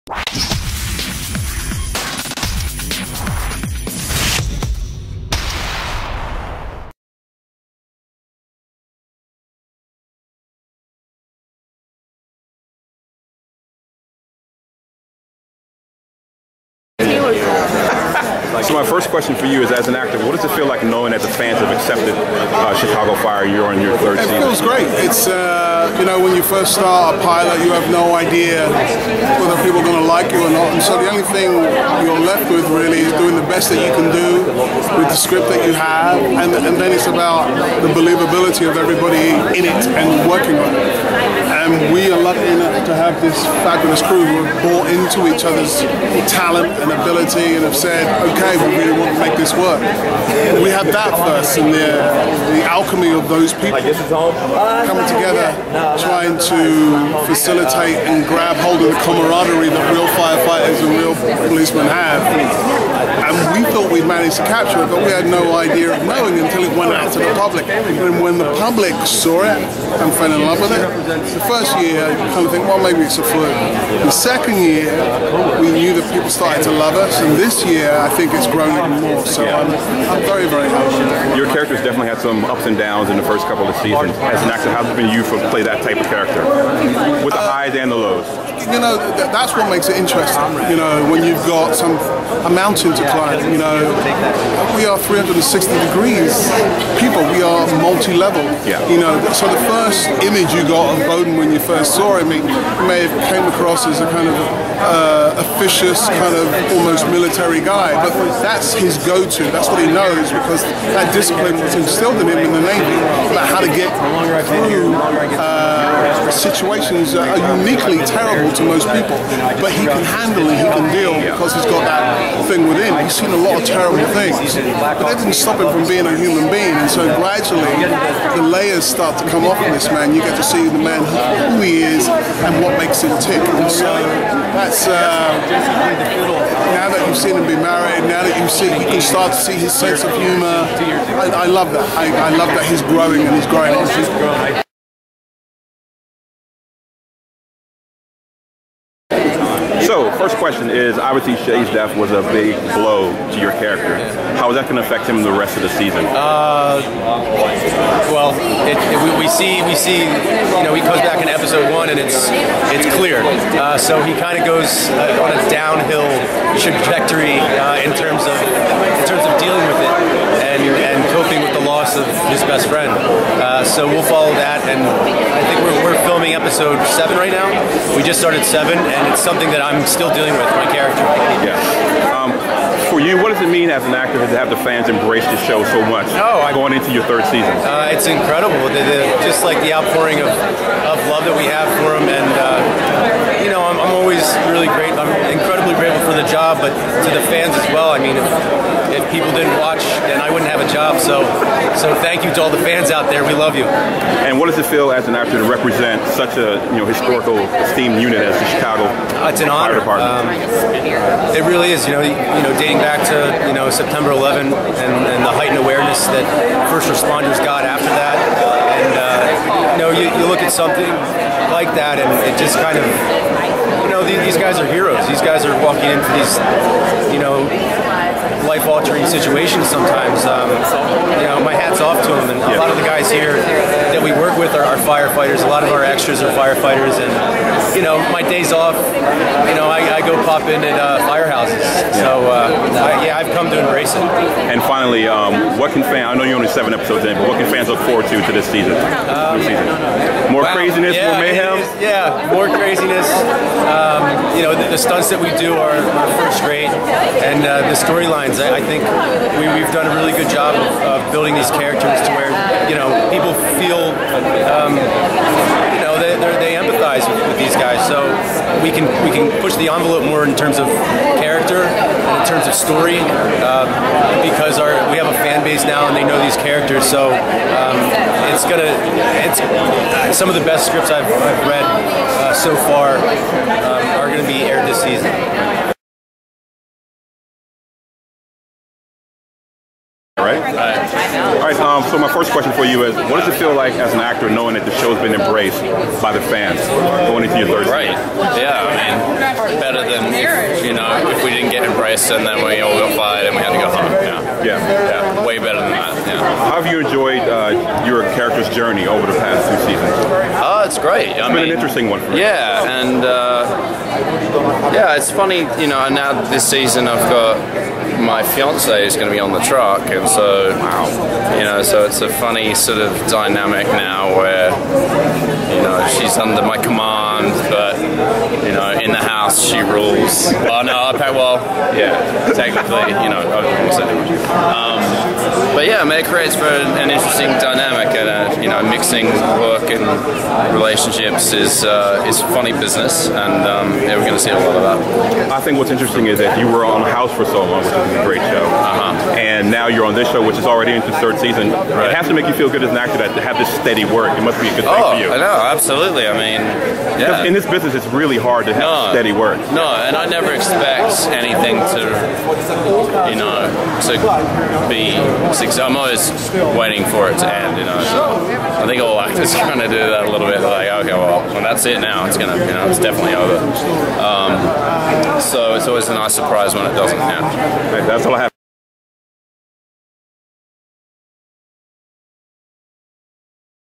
Set Visit - Chicago Fire Cast Interview
We visit the sets of Chicago PD and Chicago Fire. In this video, you will see our interviews with several cast members from Chicago Fire.